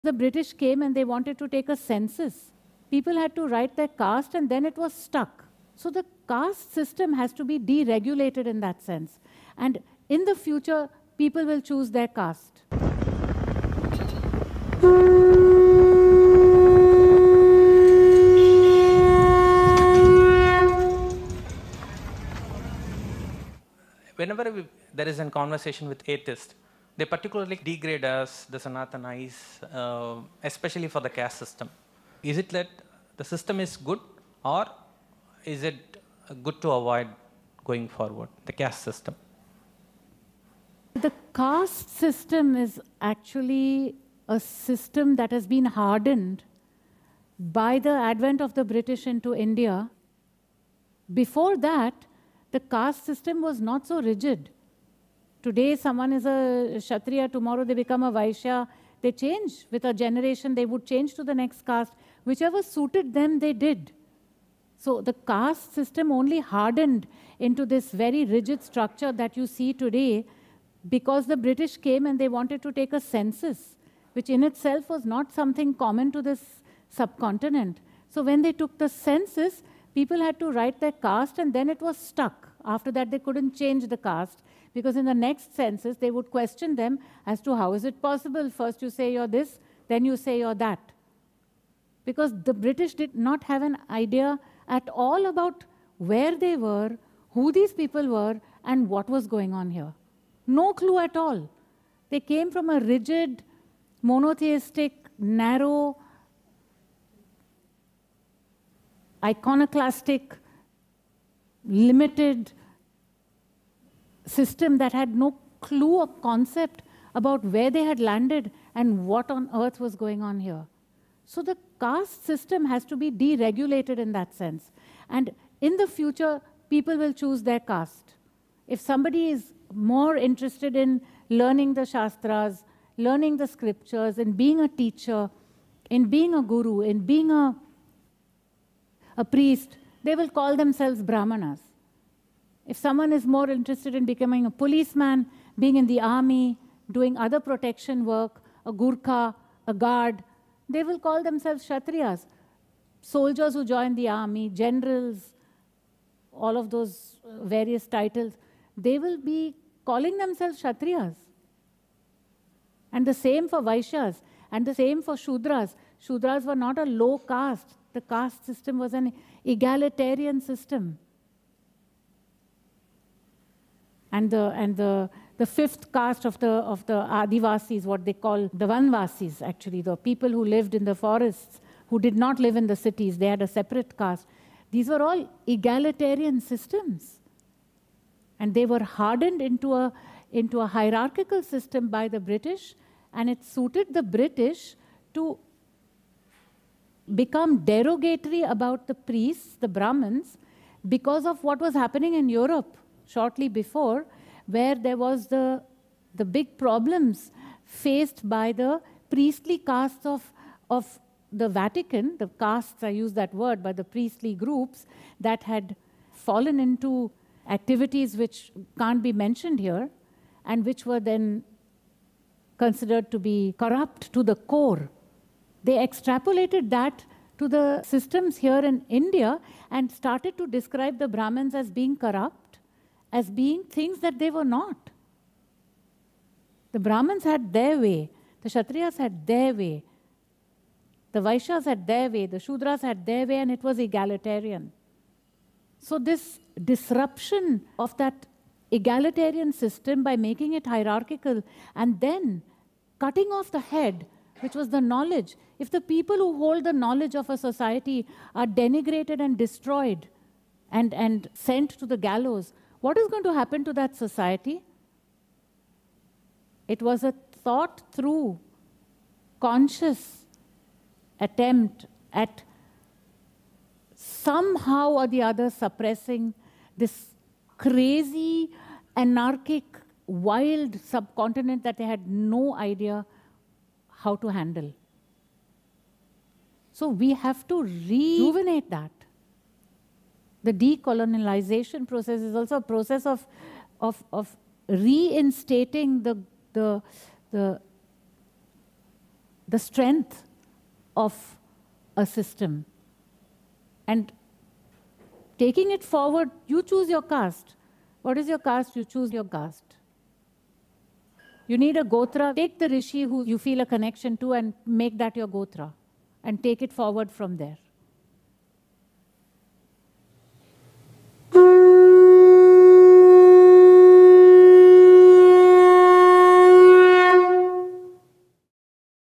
Maharishikaa | What is caste system? From British corruption to ancient Sanatani Hindu strength!
What is caste system? Maharishikaa reveals to Nambiraja a new way to approach the caste system. Having grown up as a Sanatani - a Hindu - Nambiraja experiences criticism about his religion especially for the caste system. He asks if the caste system is best done away with.

In this groundbreaking answer, the Maharishikaa explains how the caste system as it is known today is a result of a corruption by the British. What used to be a dynamic egalitarian system, allowing caste changes from one generation to the next, was turned into a rigid and hierarchical one due to the call for a census by the British rulers of India.
Fearlessly the Maharishikaa calls for a deregulation of this ossified system, inviting a future where people will choose their caste and thus reconnect with the strength of this ancient Sanatani Dharmic system.